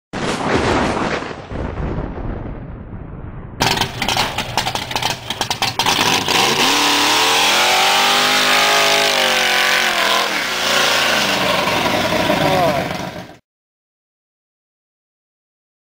Datsun 240Z Burnout
What a 327 with 600 HP does to a 240Z. there is another video on my account also of this car